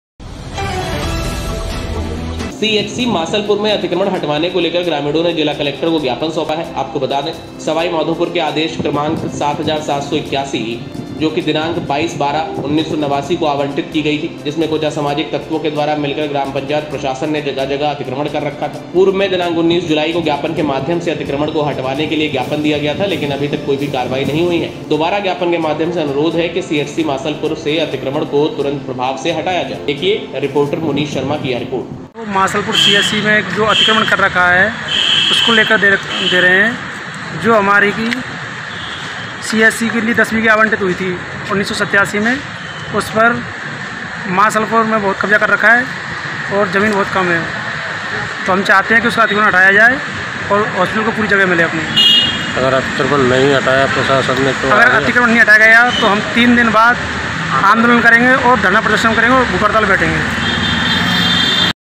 सीएचसी मासलपुर से अतिक्रमण हटवाने को लेकर ग्रामीणों ने जिला कलेक्टर को दिया ज्ञापन@happy15august
मुनेश शर्मा करौली